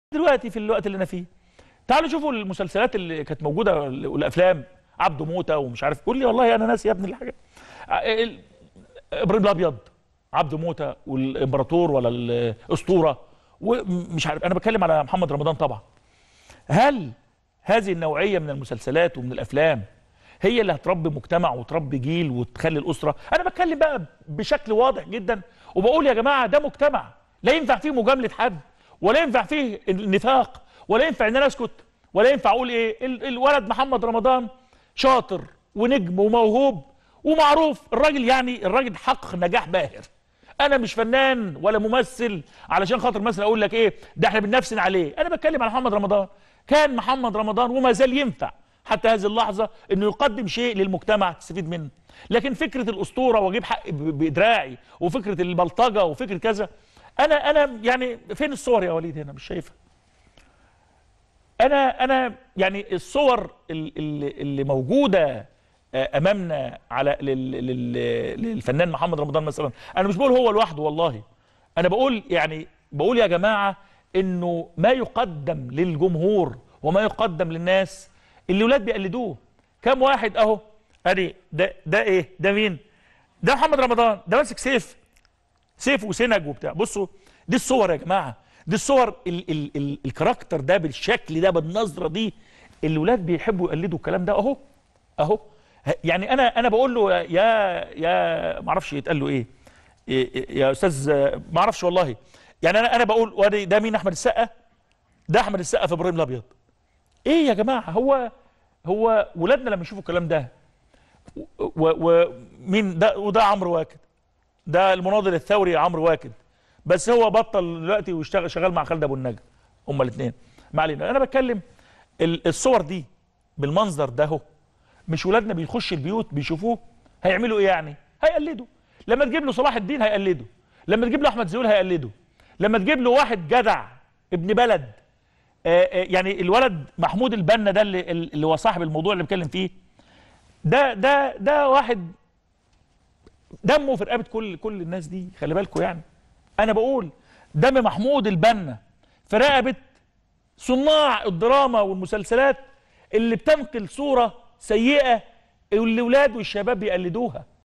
الديهي لـ محمد رمضان وصناع أفلام البلطجة "عبده موته وإبراهيم الأبيض : دم "محمود البنا" في رقبتكم
الديهي لصناع أفلام البلطجة "عبده موته وإبراهيم الأبيض والأسطورة : دم "محمود البنا" في رقبتكم